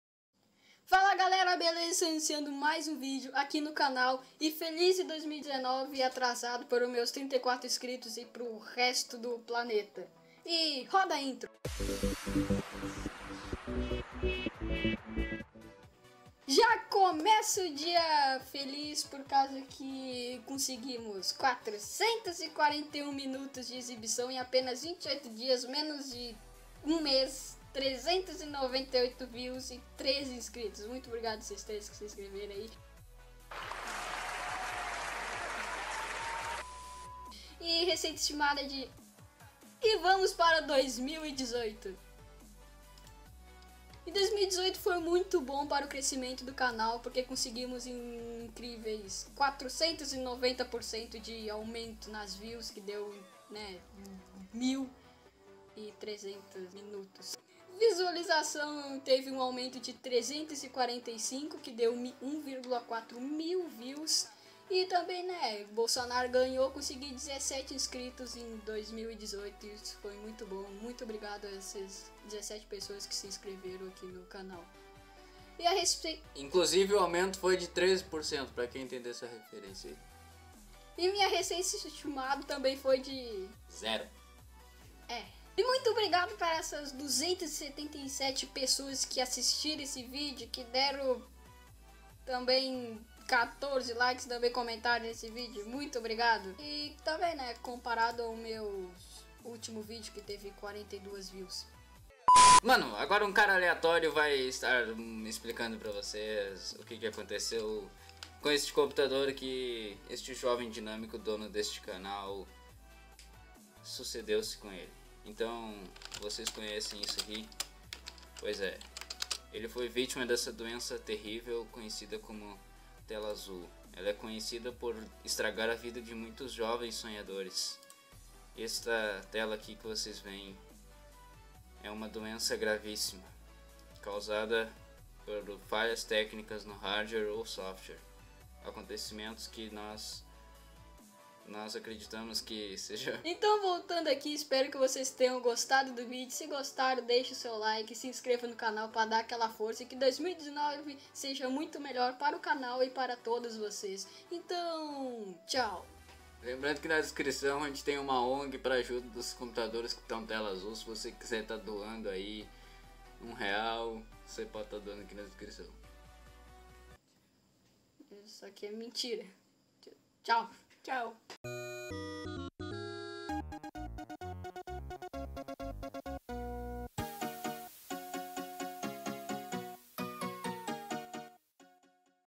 2018:O ano em que o canal "explodiu"
feliz 2019 atrasado para todos vocês, e neste vídeo estarei mostrando como 2018 foi um ano muito bom para o crescimento do canal.